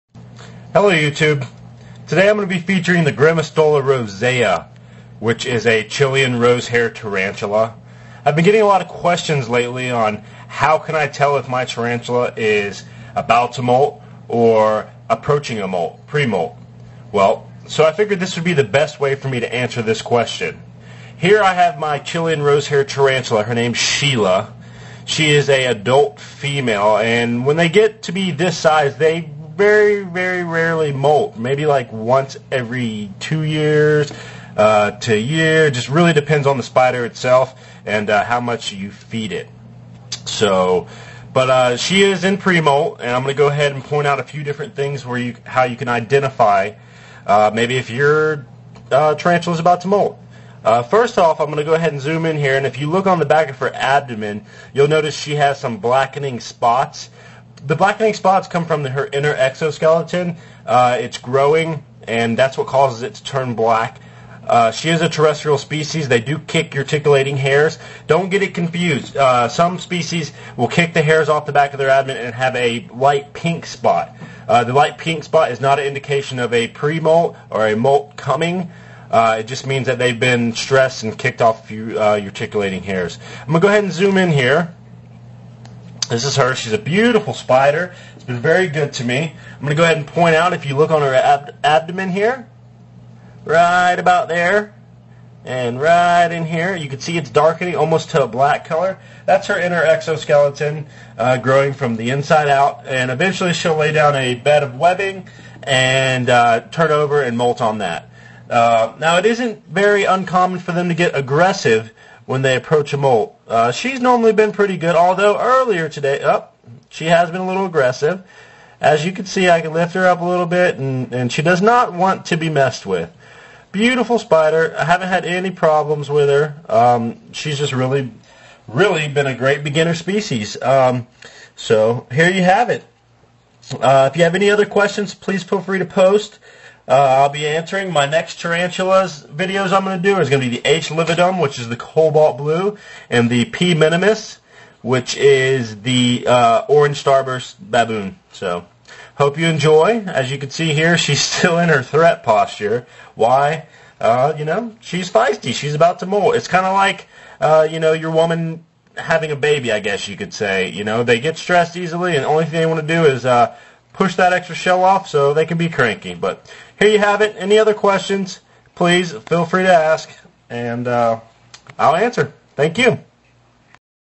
Chilean Rose Hair Tarantula
This is a G Rosea Tarantula and a few ways to tell if you have a tarantula that is near a moult.